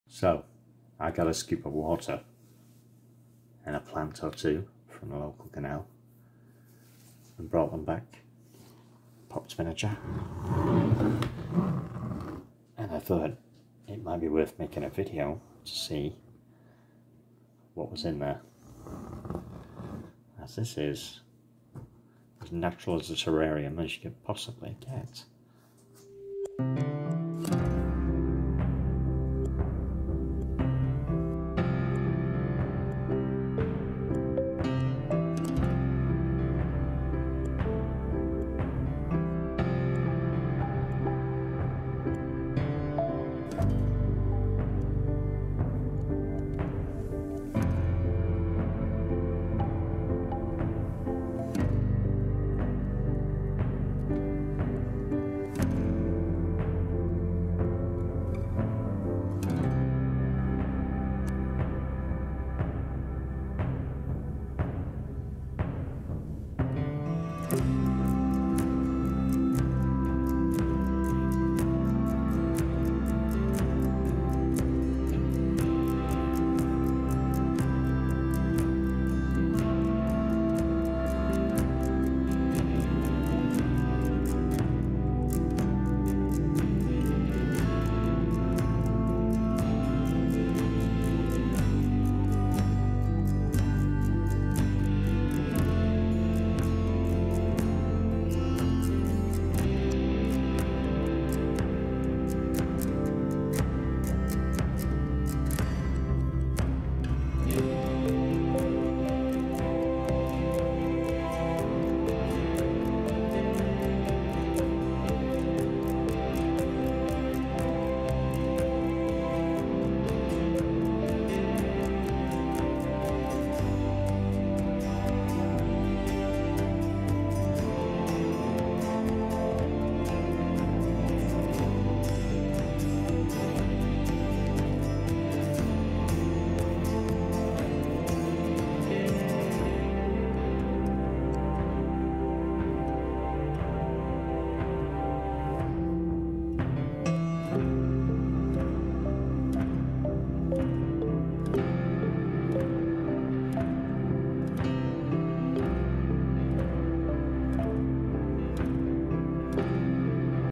Water World - Enclosed Ecology
I collected a handful of plants from a local waterway and put some in a jar with water and some reptile sand. The little world that I saw made me make this very short but interesting video of the creatures within it.

As this is not my area of knowledge, please, feel free to comment and let me know what they are and I will do a follow up video in a week or so to see what has happened.

Amphipods and water louse are present.

Please, note;
Since this quick video was released a lot more animals have emerged. A dragonfly nymph appeared that had been hiding in the plants that I hadn't noticed before making the video and eggs have been laid on the glass and plant leaves by...something :)